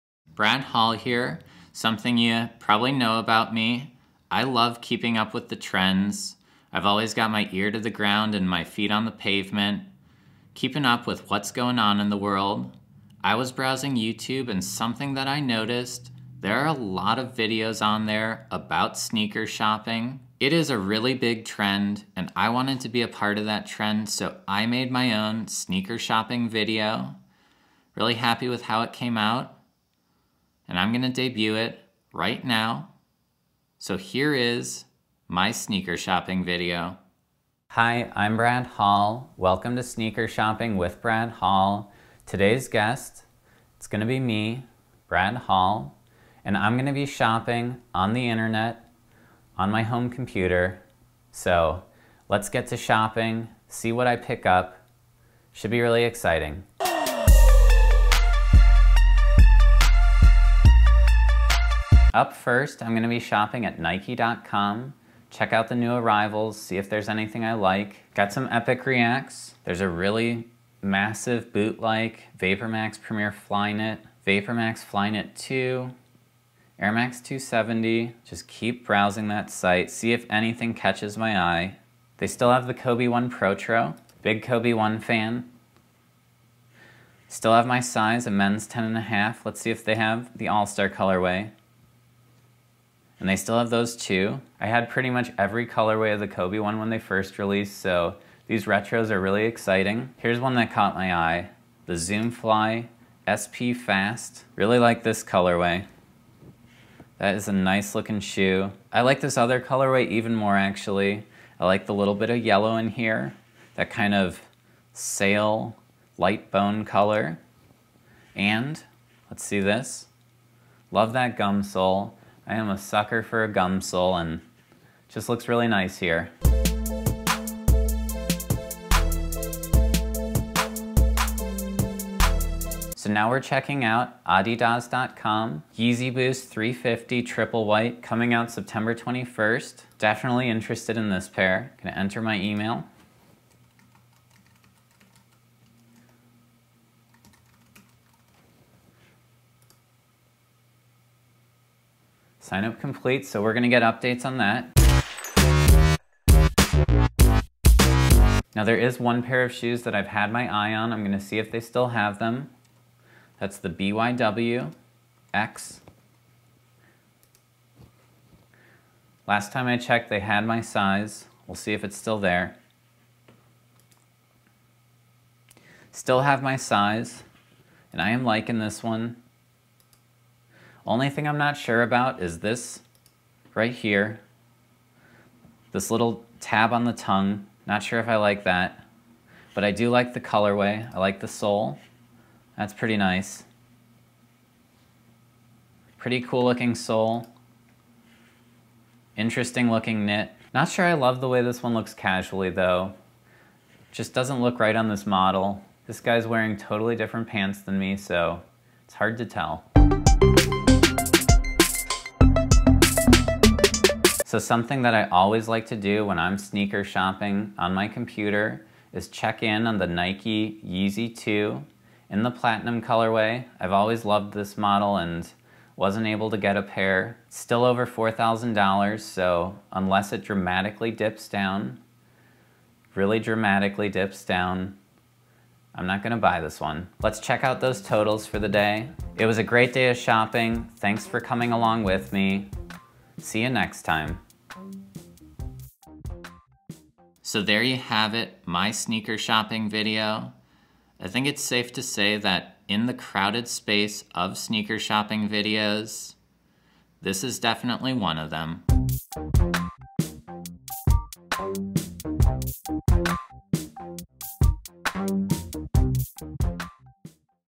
Sneaker Shopping with Brad Hall
Sneaker shopping videos are all the rage—from Complex's "Sneaker Shopping" series to a bunch of other versions of that—so I (Brad Hall) decided to hop on the trend and make one. In this episode, you'll see me shopping for Nike sneakers such as the Zoom Fly SP Fast, adidas sneakers such as the Yeezy Boost 350 V2 "Triple White," and even rare sneakers such as the Nike Yeezy 2. Enjoy watching me shop for sneakers on this episode of "Sneaker Shopping with Brad Hall."

-Brad Hall